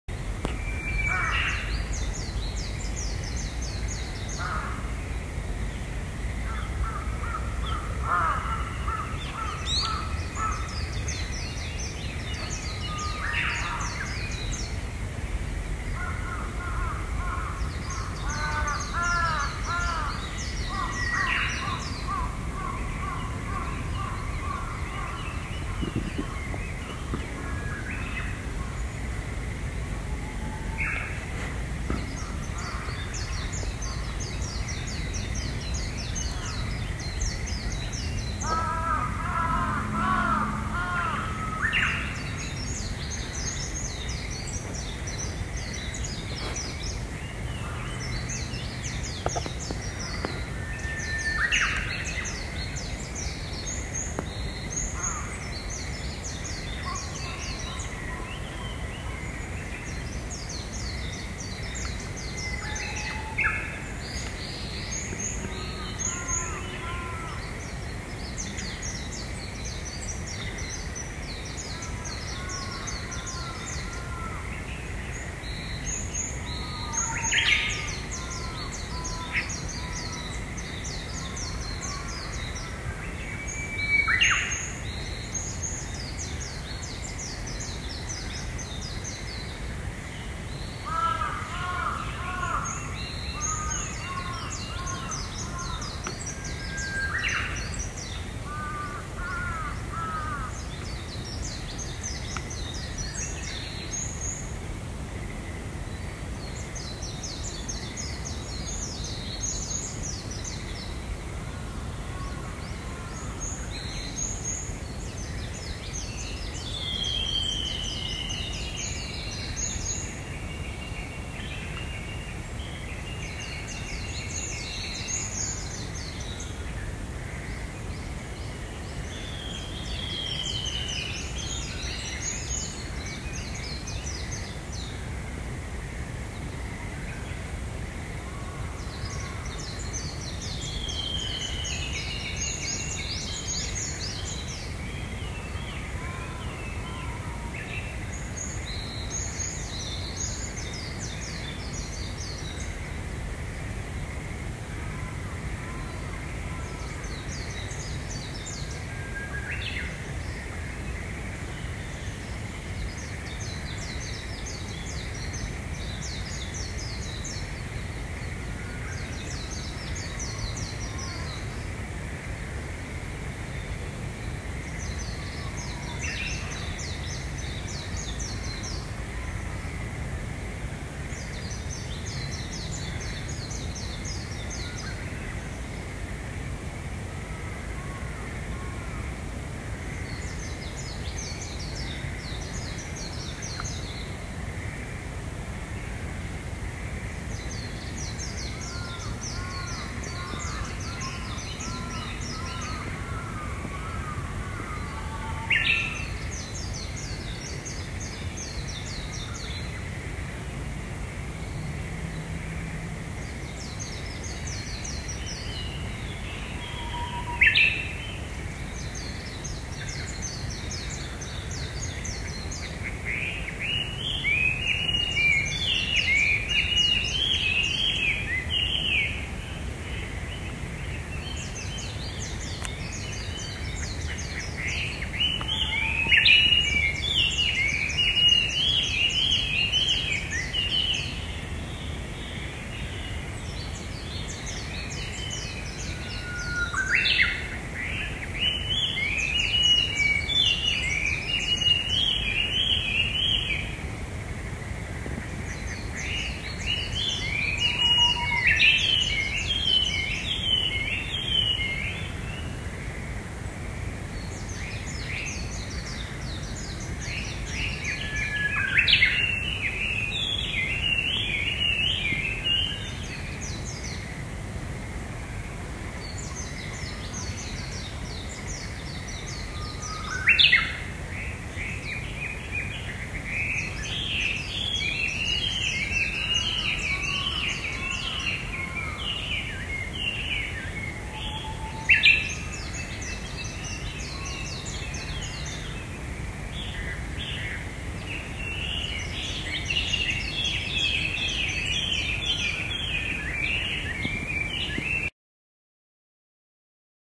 【自然音】日の出とともにさえずり始める鳥
せっかくなので朝早くから撮影してみました

toki! mi Osumi Akari!
私の興味のあることについて気の向くままに動画を投稿しています
もしこの動画が良ければ、興味のありそうな方に共有してあげてください！